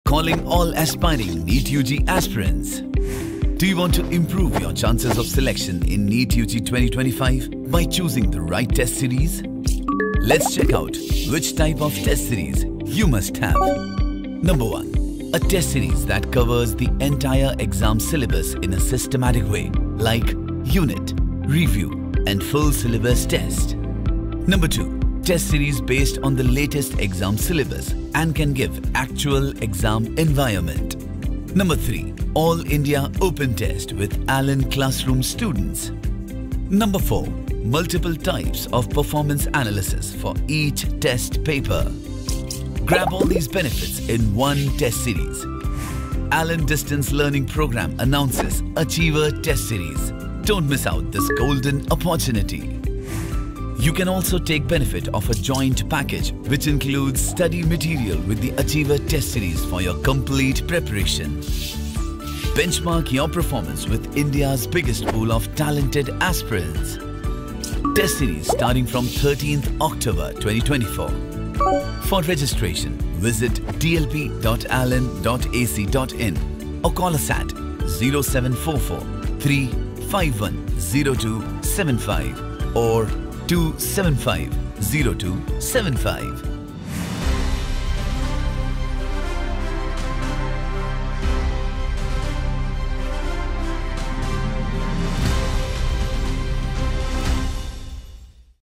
🚀 Achieve Your Dream Rank in NEET 2025 with ALLEN DLP Achiever Test Series
🚀  Your dream rank in NEET (UG) 2025 is just a series away!
🏅   Achieve your NEET 2025 dreams with the ALLEN DLP Achiever Test Series.
🗓️ Starts on 13th October 2024